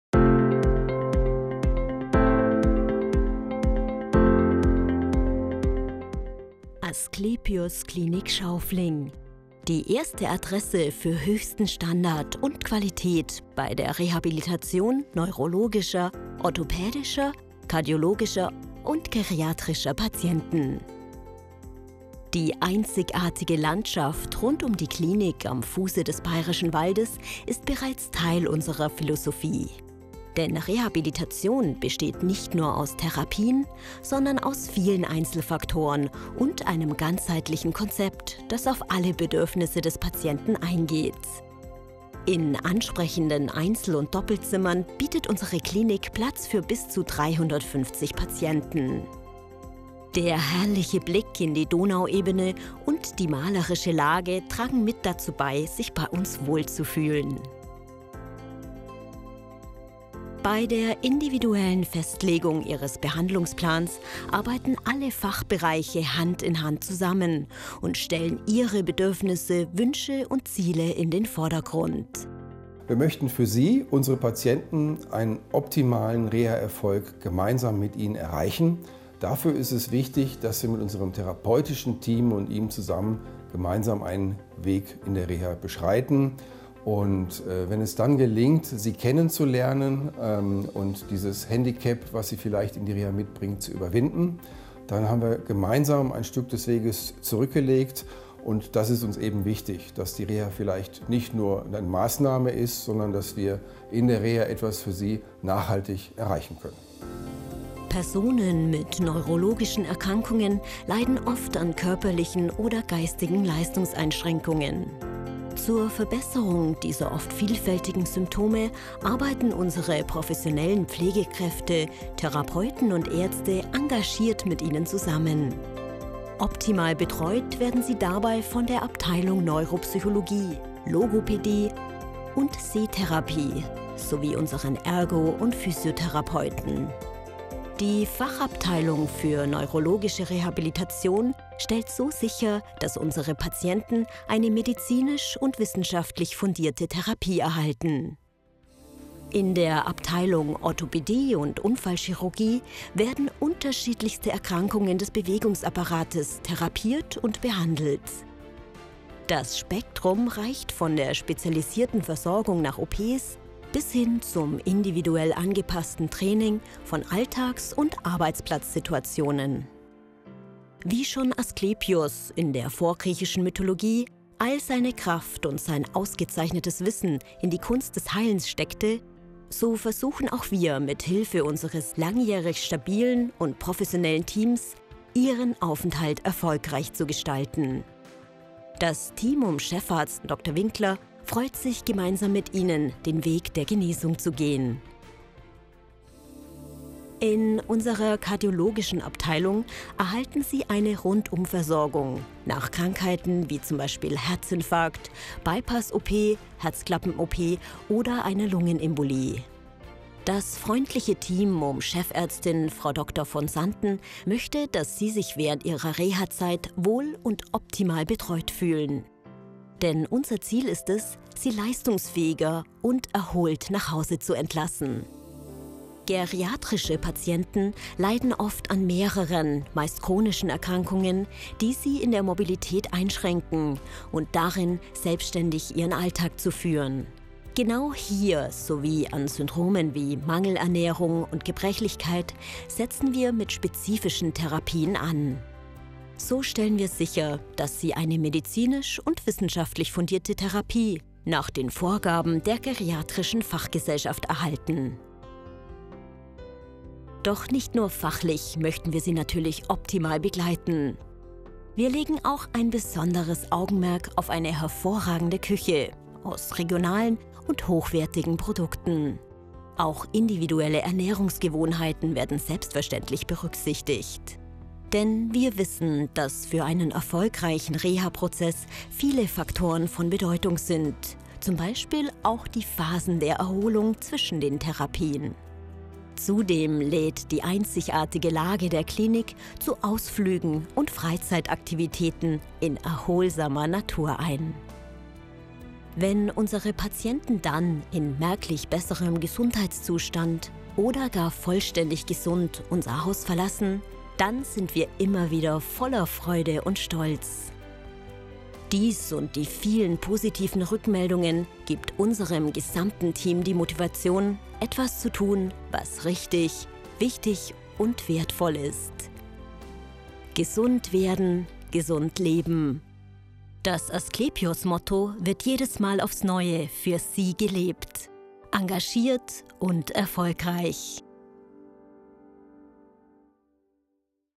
Die Asklepios Klink Schaufling - erste Adresse bei der Rehabilitation
Rehabilitation für neurologische, orthopädische, kardiologische und geriatrische Patienten in einzigartiger Landschaft und Atmosphäre nach der Devise "Gesund werden – gesund leben im schönen Bayerischen Wald“.